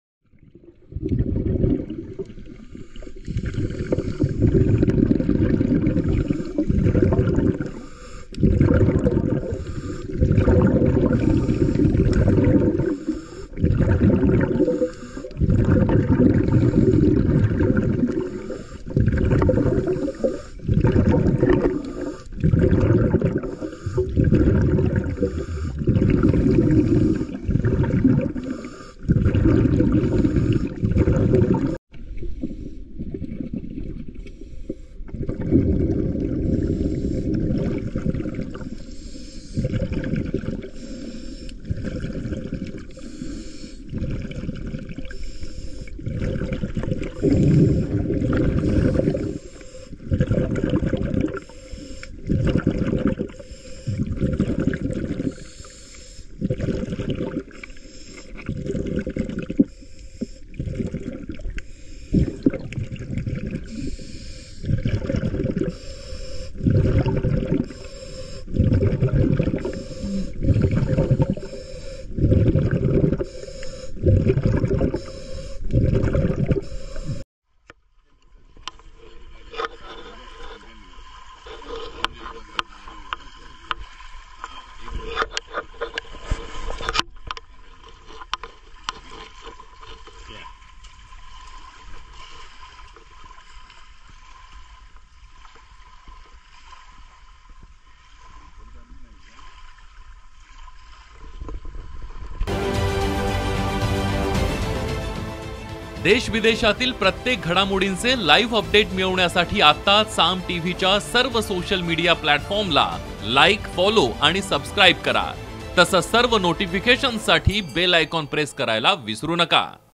Ramdas Athwale In Lakshadweep | मोदींपाठोपाठआता रामदास आठवले पोहचले लक्षद्वीपला | Marathi News
Ramdas Athwale In Lakshadweep | पंतप्रधान नरेंद्र मोदी यांनी गेल्या काही महिन्यांपूर्वी लक्षद्वीपचा दौरा केला. त्यांच्या पाठोपाठ आता केंद्रीय मंत्री रामादास आठवले हे लक्षद्वीपला पोहटलेत. यावेळी त्यांनी स्कुबा डायव्हिंग केलं. | prd